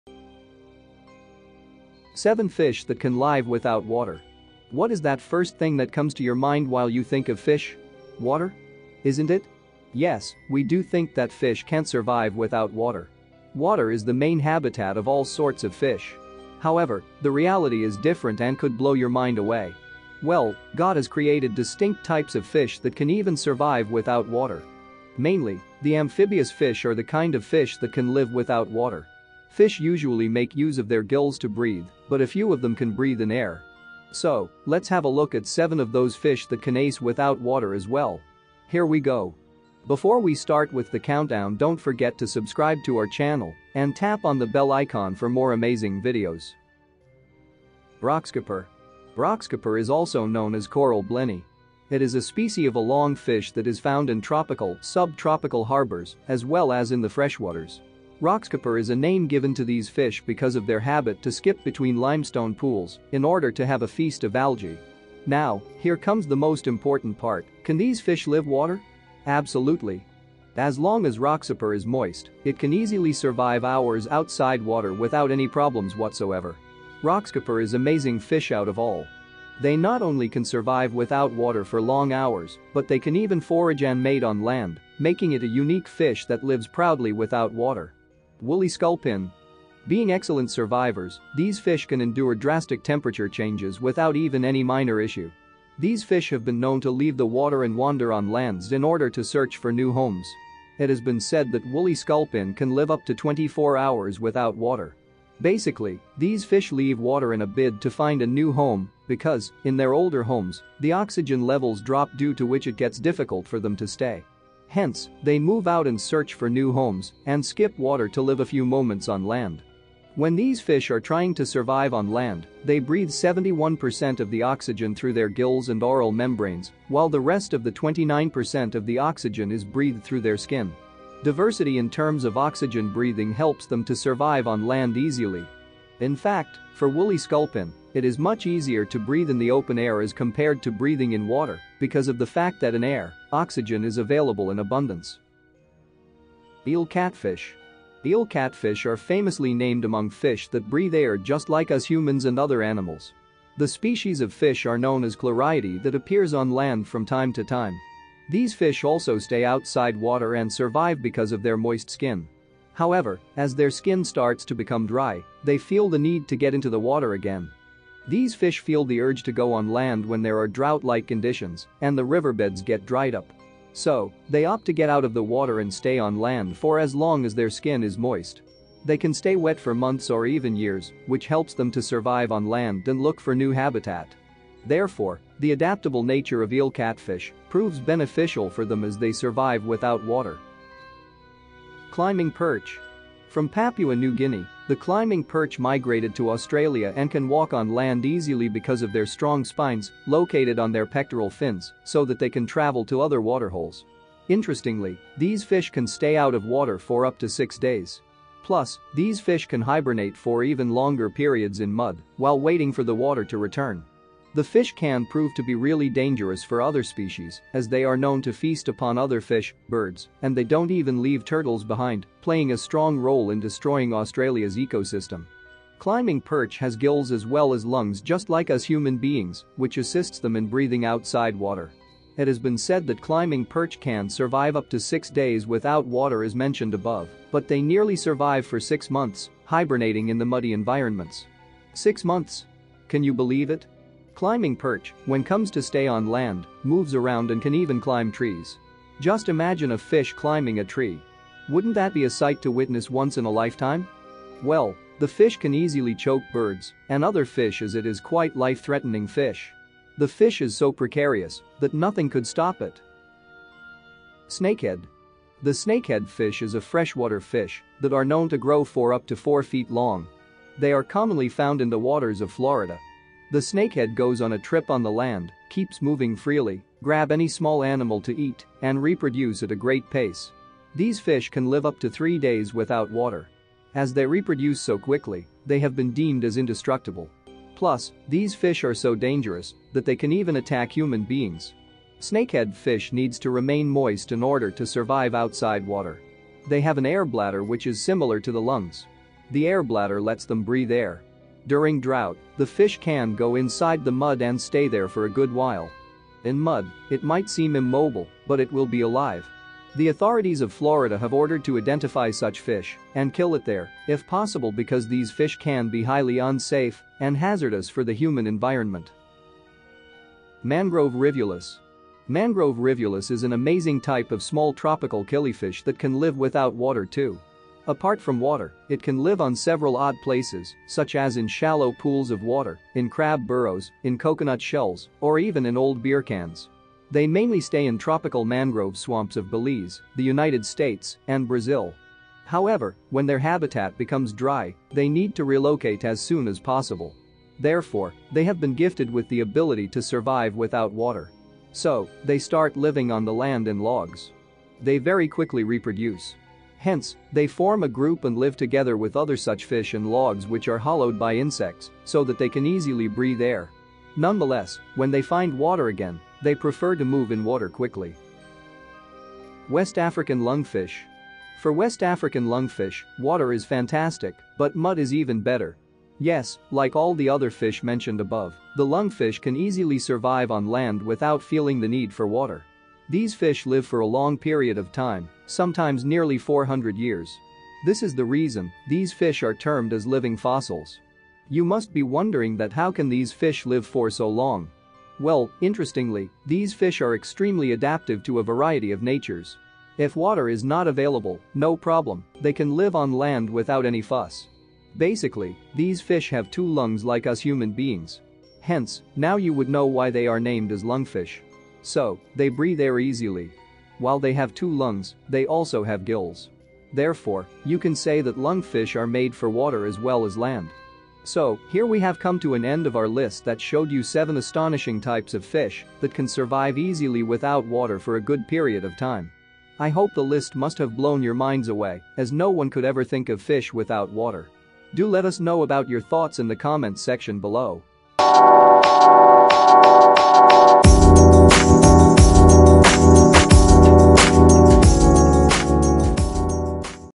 7 Fish That Can Live Without Water
What is that first thing that comes to your mind while you think of fish? Water? Isn’t it?

Yes, we do think that fish can’t survive without water. Water is the main habitat of all sorts of fish. However, the reality is different and could blow your mind away. Well, God has created distinct types of fish that can even survive without water!

Mainly, the amphibious fish are the kind of fish that can live without water. Fish usually make use of their gills to breathe but a few of them can breathe in air.

So, let’s have a look at 7 of those fish that can ace without water as well. Here we go!